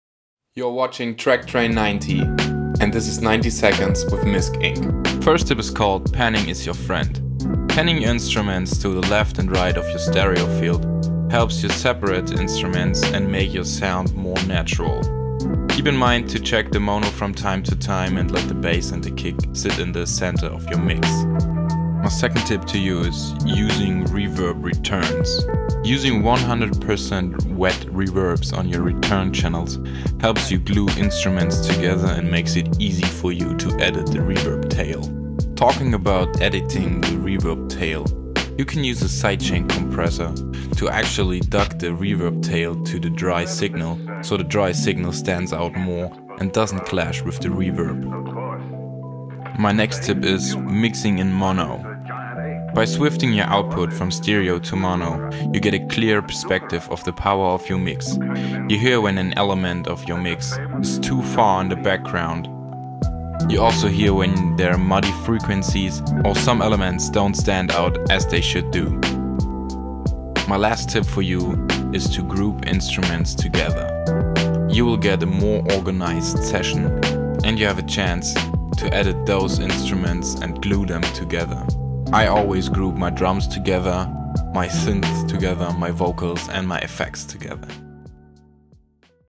5 Tips For Mixing Lo Fi Hip Hop In 90 Seconds With Misc Inc
Mixing is one of the most challenging tasks in the life of a producer. Misc Inc shows you 5 mixing tips on how to achieve a cleaner and louder mix.